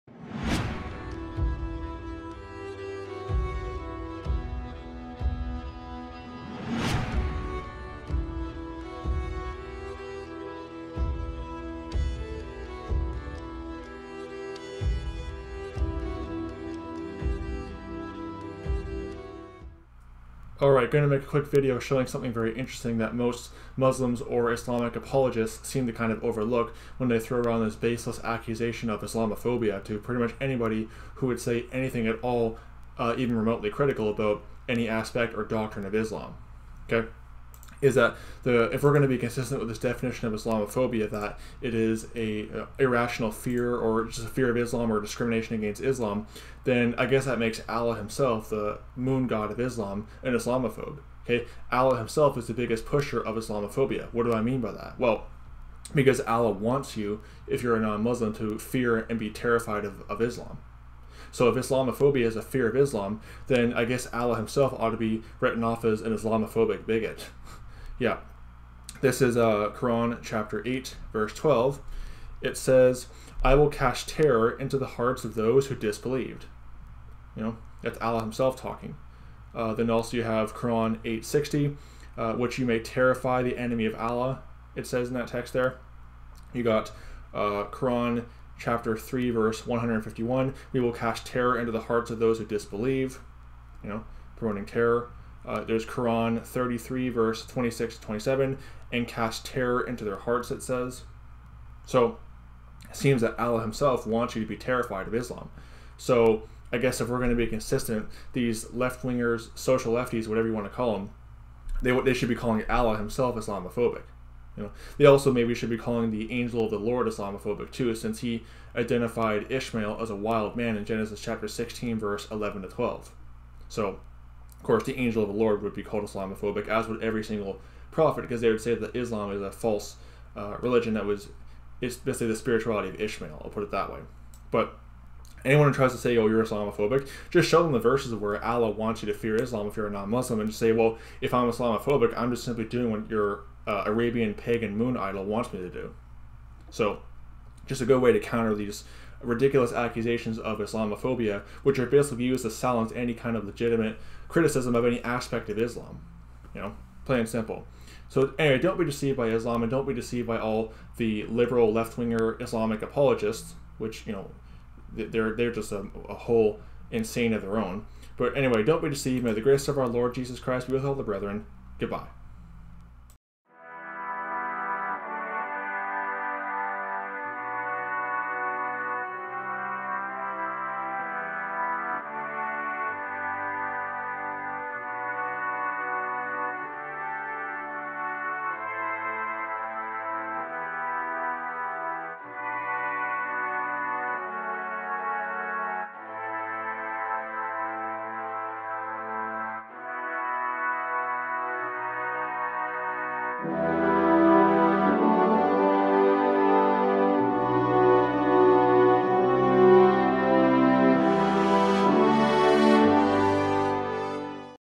The Islamic Moon-Idol Allah Is The Biggest Pusher Of "Islamophobia"
If you are a non-Muslim and you are “Islamophobic” then Muslims have zero reasons to get upset at you!